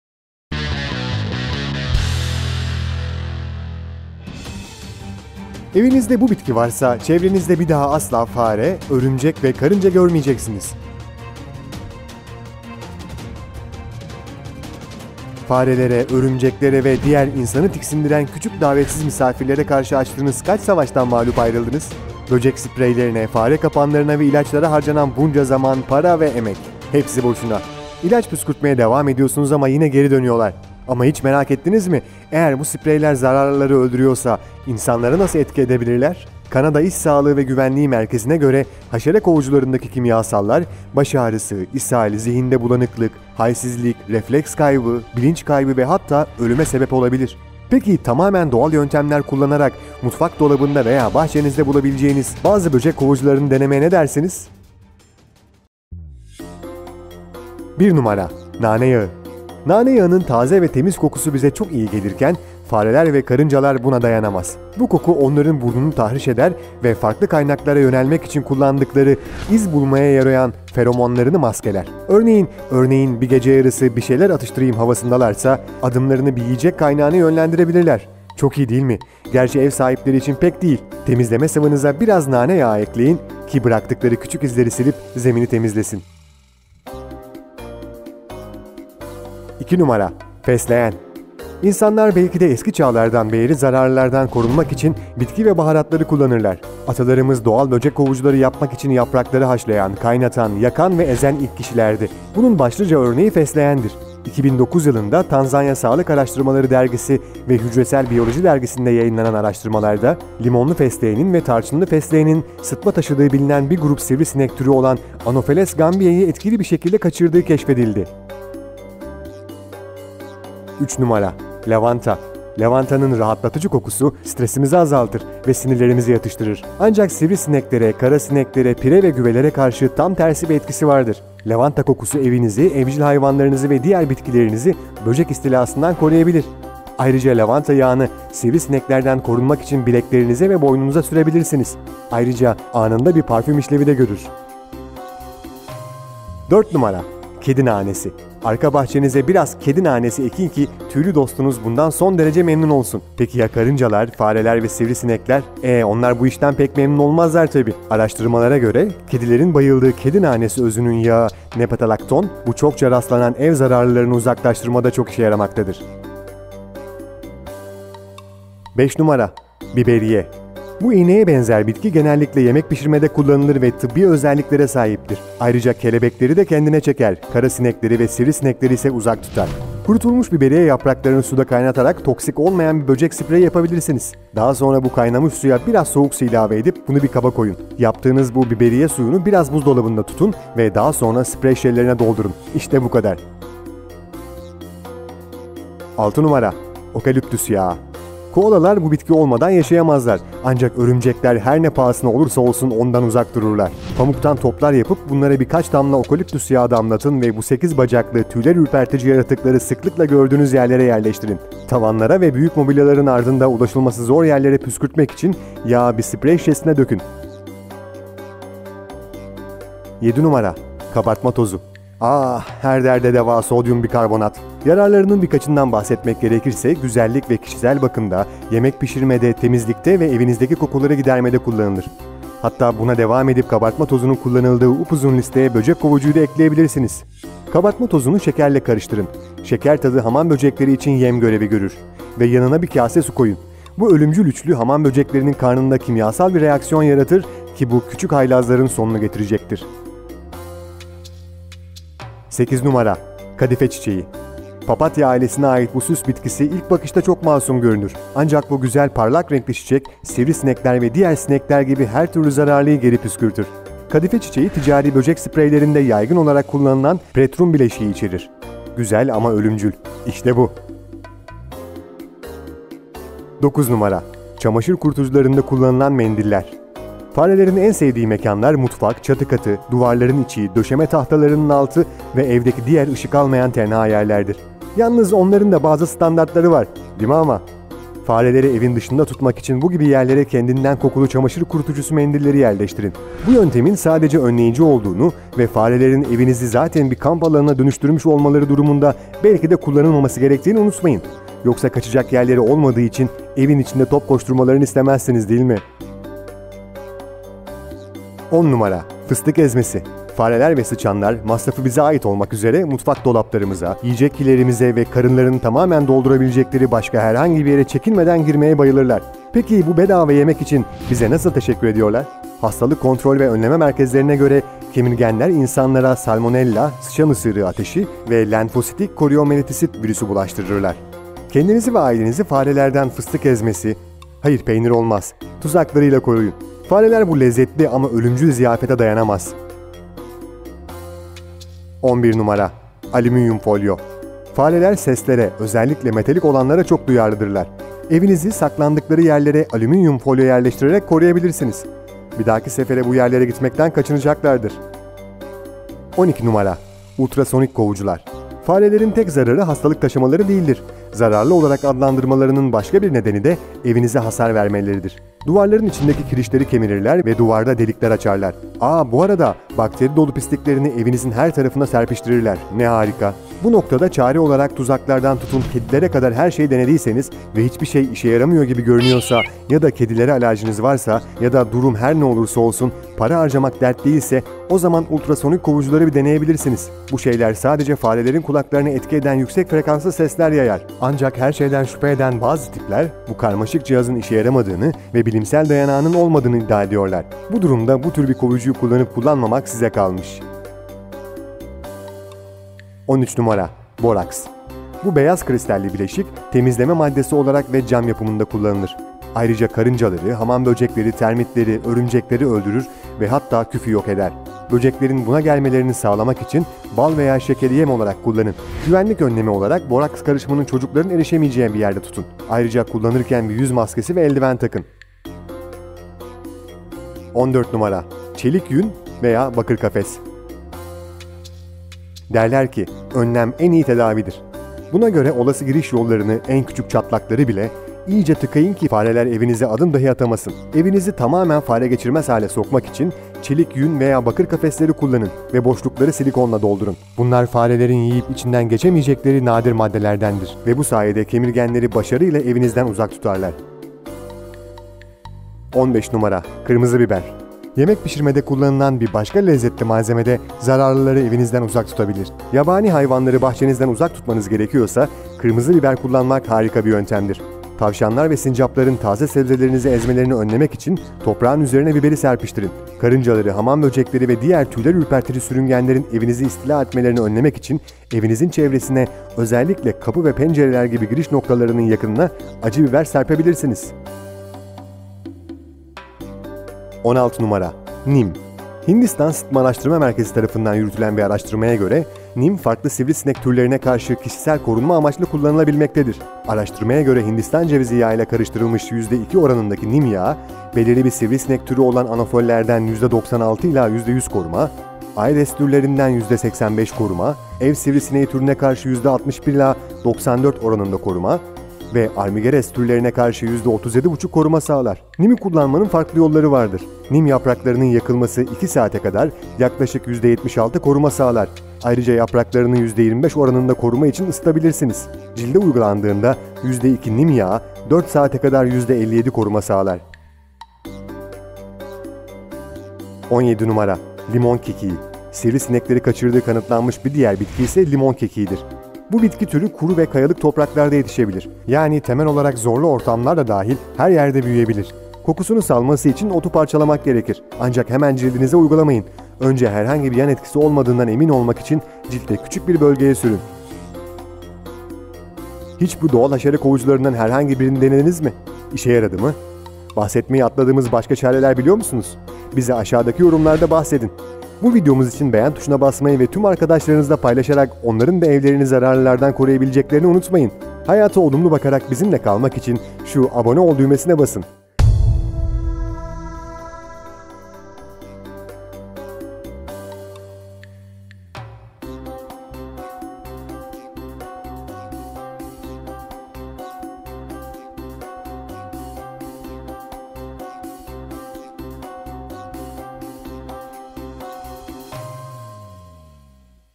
Evinizde Bu Bitki Varsa, Çevrenizde Bir Daha Asla Fare, Örümcek Ve Karınca Görmeyeceksiniz
Fare, Örümcek Ve Karıncalardan Nasıl Kurtulunur. Kimyasal ilaçlarla kendinize zarar vermeden evdeki zararlıları nasıl uzak tutarsınız? Kanada İş Sağlığı ve Güvenliği Merkezine göre, haşere kovucu kimyasallar baş ağrısı, ishal, zihinde bulanıklık, halsizlik, refleks kayıpları, bilinç kaybı ve hatta ölüme sebep olabilir.
Pekiyi tamamen doğal yöntemler kullanarak mutfak dolabında veya bahçenizde bulabileceğiniz bazı böcek kovucularını denemeye ne dersiniz? Örümcekleri uzak tutmak için okaliptüs yağını, karınca izlerini silmek için nane yağını, sivri sinek ve kara sinekler gibi her tür zararlı için kadife çiçeğini, fareleri evden uzak tutmak için alüminyum folyo veya kendinden kokulu kurutma makinesi mendillerini deneyebilirsiniz.

Zaman bilgileri:
Nane yağı nasıl kullanılır 1:03
Fesleğen nasıl kullanılır 1:36
Lavanta nasıl kullanılır 2:13
Kedi nanesi nasıl kullanılır 2:40
Biberiye nasıl kullanılır 3:06
Okaliptüs yağı nasıl kullanılır 3:34
Kabartma tozu nasıl kullanılır 4:02
Kadife çiçeği  nasıl 4:37
Kurutucu makinası mendilleri nasıl kullanılır 5:01
Fıstık ezmesi nasıl kullanılır 5:34
Alüminyum folyo nasıl kullanılır 6:11
Ultrasonik kovucular nasıl kullanılır 6:28
Boraks nasıl kullanılır 7:21
Çelik yünü veya bakır kafes nasıl kullanılır 7:50
Kırmızı biber nasıl kullanılır  8:19
Nim nasıl kullanılır 8:51
Limon kekiği nasıl kullanılır 9:47

Özetler:
- Temizleme sıvısına biraz nane yağı ekleyin ve karınca izlerini silmek için zemini bununla paspaslayın.
-Limon fesleğeni ve tarçın fesleğeni, sıtma taşıdığı bilinen bir grup sivrisinek türü olan Anopheles gambiae yi etkili bir şekilde uzak tutmaya yarar.
- Lavanta kokusu evinizi, evcil hayvanlarınızı ve diğer bitkilerinizi böcek istilasına karşı koruyabilir.
- Araştırmalara göre, kedilerin bayıldığı kedi nanesi özünün yağı  nepetalactone, bu çokça rastlanan ev zararlılarını uzaklaştırmada çok işe yarar.
- Biberiye, yaygın olarak yemek pişirmede kullanılır ve tıbbi özelliklere sahiptir. Ayrıca kelebekleri de kendine çekerken, kara sinekleri ve sivrisinekleri uzak tutar.
- Pamuk toplarına birkaç damla okaliptüs özül yağı dökün ve bunları örümcekleri sık gördüğünüz yerlere koyun.
- Kabartma tozuyla şeker ve yanına bir kase su koyun. Bu ölümcül üçlü, hamamböceklerinin karnında kimyasal bir reaksiyon yaratır, ki bu küçük haylazların sonunu getirir.
- Kadife çiçeği, ticari böcek spreylerinde yaygın olarak kullanılan piretrum bileşiği içerir.
- Bu yerlere fareleri uzak  tutmak için kendinden kokulu çamaşır kurutucusu mendillerinden koyun.
- Fare ve sıçanlara karşı fıstık ezmesiyle tuzaklar kurarak kendinizi ve ailenizi koruyun.
- Saklandıkları yerlere alüminyum folyo yerleştirerek evinizi farelerden koruyun.
- Para harcamayı göze alıyorsanız ultrasonik kovucuları deneyin. Bu şeyler sadece farelerin kulaklarının duyarlı olduğu yüksek frekanslı sesler yayarlar.
-Boraks, temizlik maddesi olarak ve cam yapımında kullanılır. Ayrıca karıncaları, hamamböceklerini, termitleri, örümcekleri öldürebilir ve hatta küfe karşı iyi gelir.
- Evinizi farelere karşı korumak için çelik yün veya bakır kafesler kullanın ve boşlukları silikonla doldurun.
- Vahşi hayvanları bahçenizden uzak tutmanız gerekiyorsa, acı biber bunun için mükemmel bir çaredi. Tavşanlar ve sincapların taze sebzelerinizi ezmelerini önlemek için biberi toprağın üzerine serpin.
- Nim yapraklarını yakmak sivrisineklere karşı iki saate kadar yaklaşık %76 koruma sağlar. Ayrıca yapraklarını ısıtarak %25 oranında koruma sağlayabilirsiniz.
-Sivrisinekleri uzak tuttuğu kanıtlanan başka bir bitki de limonlu kekiktir. Kekiği ezin ve vücudunuzun küçük bir kısmında denedikten sonra cildinize uygulayın.